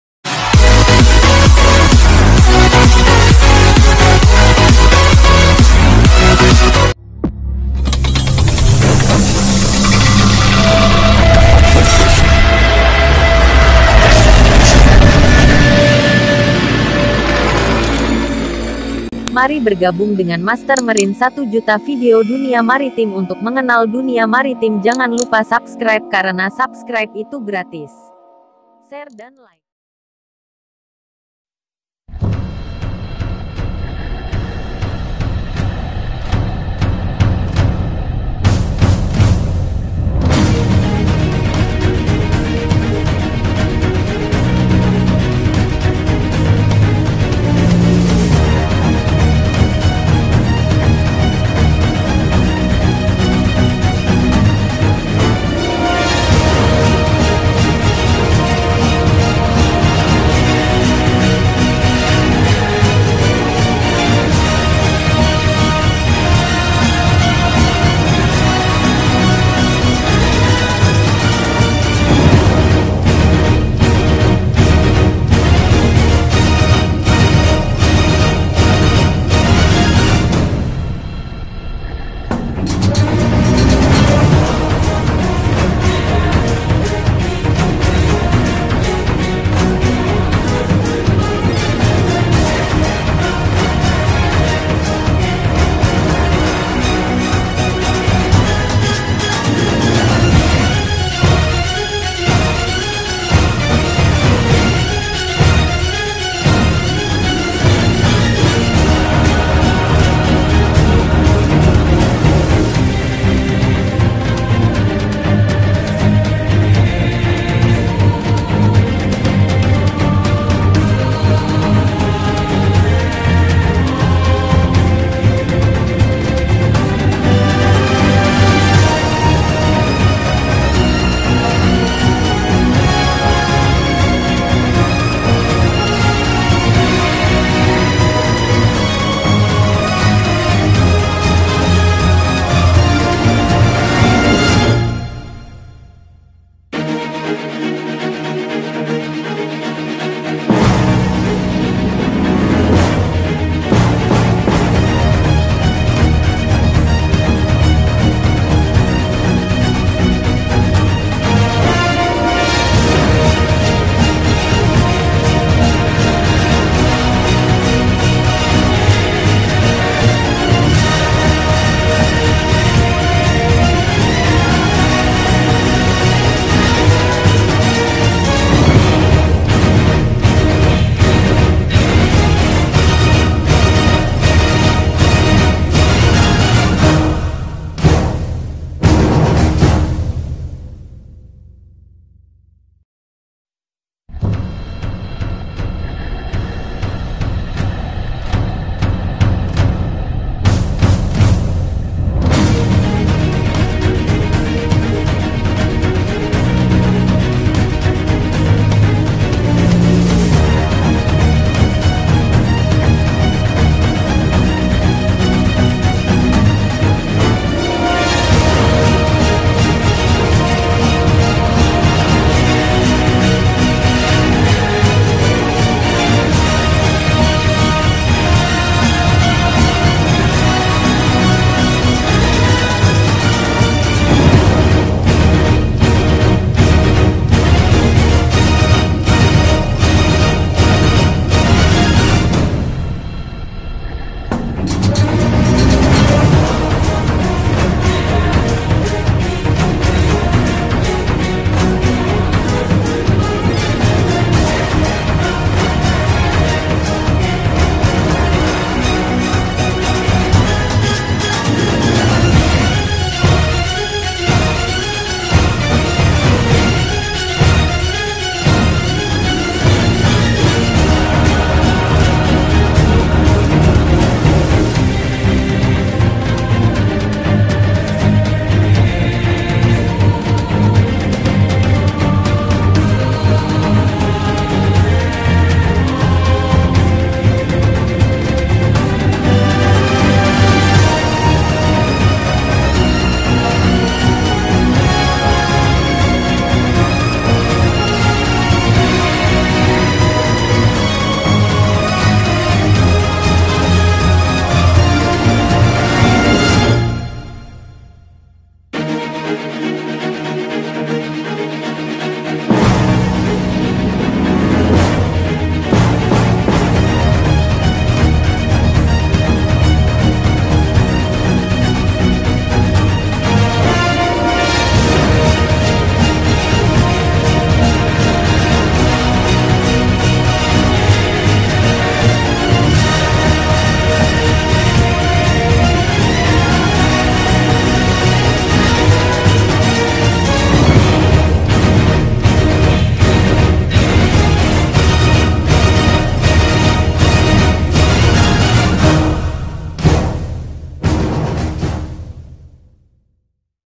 5 DAYS FULL CARGO LOADING IN SWEDEN
HAY GUYS BEGINILAH CARA PEMUATAN KAPAL CARGO.SEMOGA BERMANFAAT